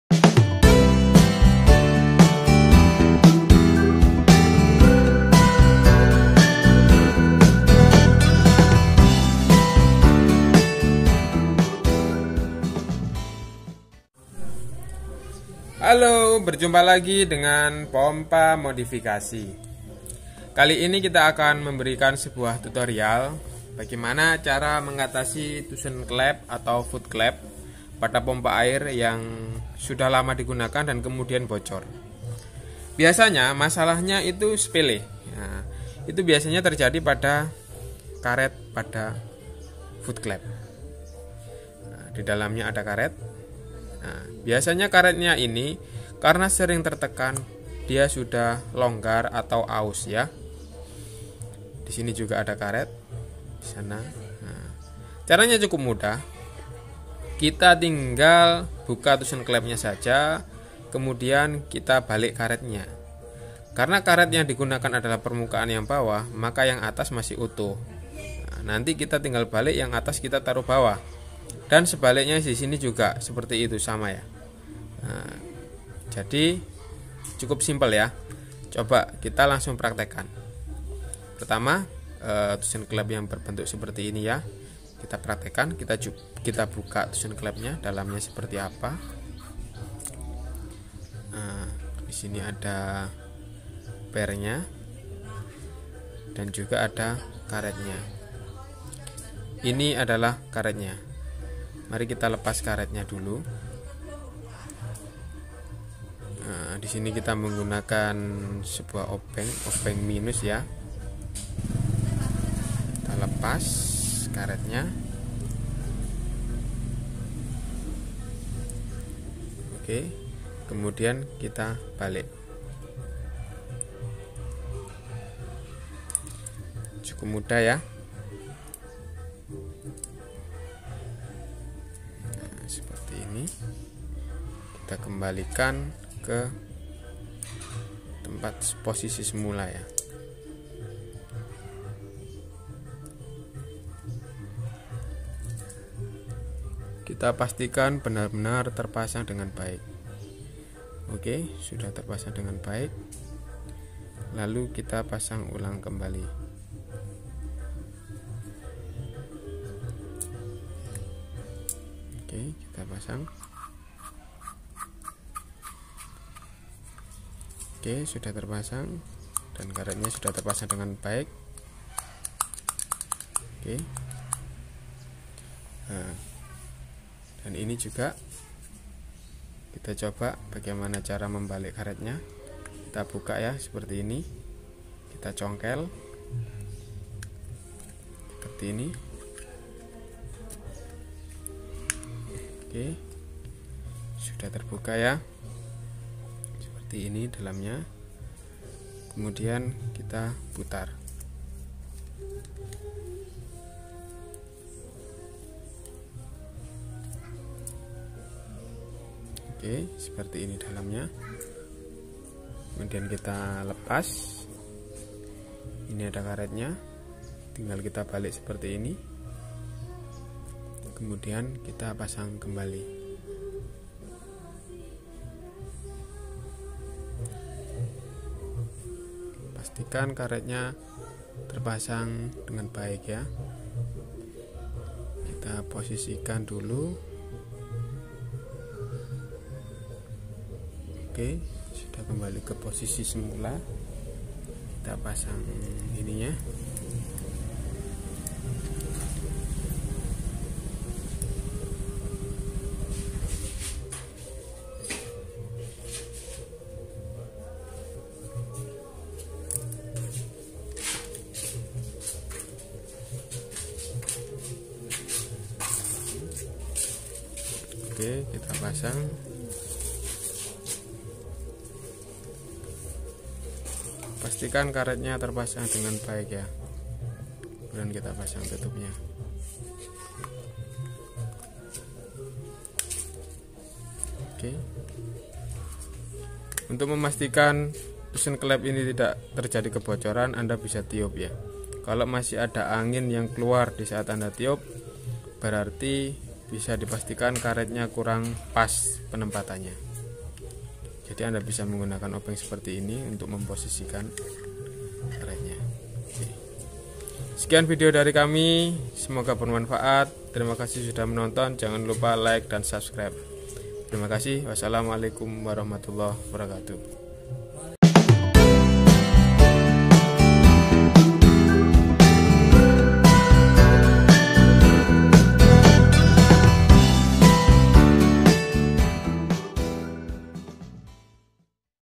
TUSEN KLEP Bocor ini SOLUSINYA
Video ini memberikan tutorial bagaimana cara mengatasi tusen klep bocor akibat pemakaian yang sudah terlalu lama. caranya cukup mudah silahkan disimak video kami terimakasih